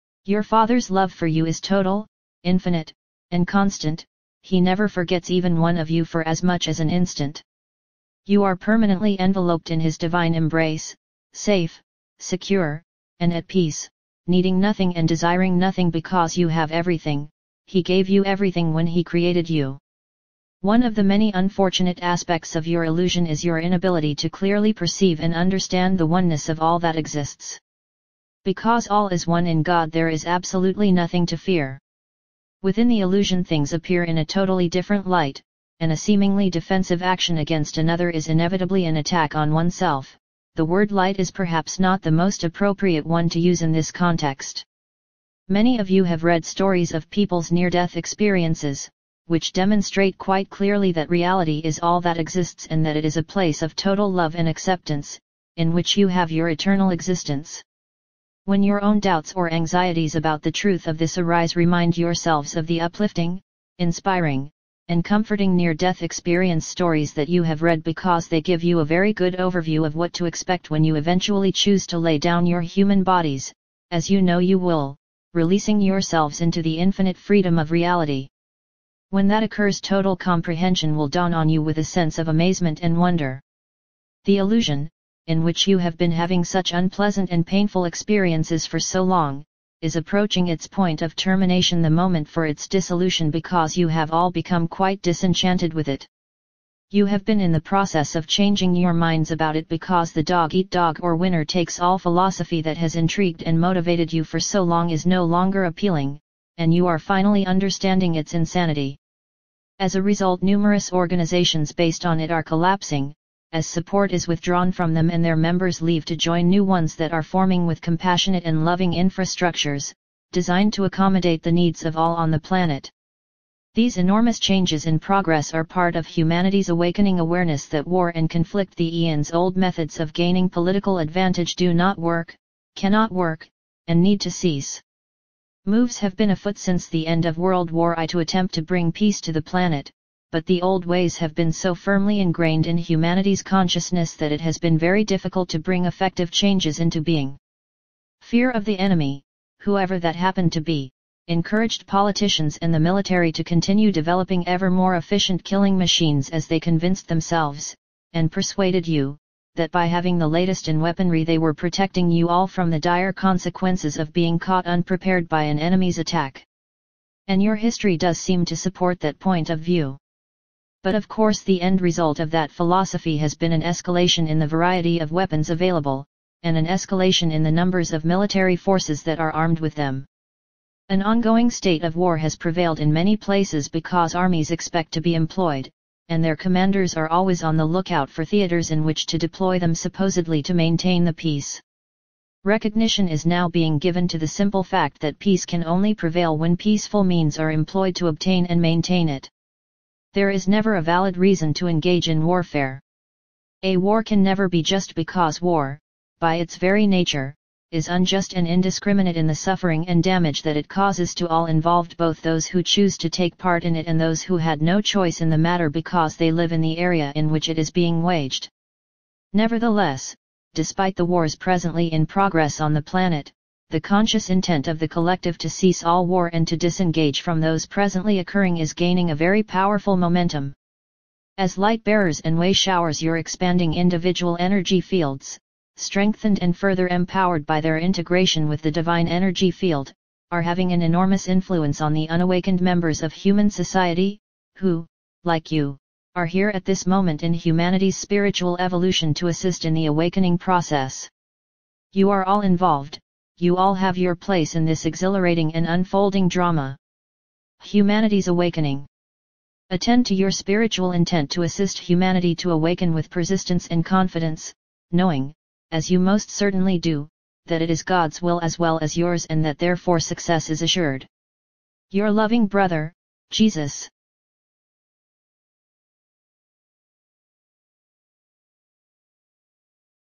Galactic Federation of Light Jesus October-31-2012
Peace can only prevail when peaceful means are employed to obtain it
October 31, 2012 by John Smallman

Your Father's Love for you is total, infinite, and constant, He never forgets even one of you for as much as an instant.  You are permanently enveloped in His divine embrace, safe, secure, and at peace, needing nothing and desiring nothing because you have everything -- He gave you everything when He created you.

One of the many unfortunate aspects of your illusion is your inability to clearly perceive and understand the oneness of all that exists.  Because all is one in God there is absolutely nothing to fear.  Within the illusion things appear in a totally different light, and a seemingly defensive action against another is inevitably an attack on oneself (the word light is perhaps not the most appropriate one to use in this context).

Many of you have read stories of people's near-death experiences (NDEs), which demonstrate quite clearly that Reality is all that exists and that it is a place of total Love and Acceptance, in which you have your eternal existence.  When your own doubts or anxieties about the truth of this arise remind yourselves of the uplifting, inspiring, and comforting NDE stories that you have read because they give you a very good overview of what to expect when you eventually choose to lay down your human bodies, as you know you will, releasing yourselves into the infinite freedom of Reality.  When that occurs total comprehension will dawn on you with a sense of amazement and wonder.

The illusion, in which you have been having such unpleasant and painful experiences for so long, is approaching its point of termination — the moment for its dissolution — because you have all become quite disenchanted with it.  You have been in the process of changing your minds about it because the "dog eat dog" or "winner takes all" philosophy that has intrigued and motivated you for so long is no longer appealing, and you are finally understanding its insanity.  As a result numerous organizations based on it are collapsing, as support is withdrawn from them and their members leave to join new ones that are forming with compassionate and loving infrastructures, designed to accommodate the needs of all on the planet.

These enormous changes in progress are part of humanity's awakening awareness that war and conflict — the eon's old methods of gaining political advantage — do not work, cannot work, and need to cease.  Moves have been afoot since the end of World War I to attempt to bring peace to the planet, but the old ways have been so firmly ingrained in humanity's consciousness that it has been very difficult to bring effective changes into being.  Fear of the "enemy," whoever that happened to be, encouraged politicians and the military to continue developing ever more efficient killing-machines as they convinced themselves, and persuaded you, that by having the latest in weaponry they were protecting you all from the dire consequences of being caught unprepared by an enemy's attack.  And your history does seem to support that point of view.

But of course the end result of that philosophy has been an escalation in the variety of weapons available, and an escalation in the numbers of military forces that are armed with them.  An ongoing state of war has prevailed in many places because armies expect to be employed, and their commanders are always on the lookout for theaters in which to deploy them -- supposedly to maintain the peace.

Recognition is now being given to the simple fact that peace can only prevail when peaceful means are employed to obtain and maintain it.  There is never a valid reason to engage in warfare.  A war can never be "just" because war, by its very nature, is unjust and indiscriminate in the suffering and damage that it causes to all involved — both those who choose to take part in it and those who had no choice in the matter because they live in the area in which it is being waged.  Nevertheless, despite the wars presently in progress on the planet, the conscious intent of the collective to cease all war and to disengage from those presently occurring is gaining a very powerful momentum.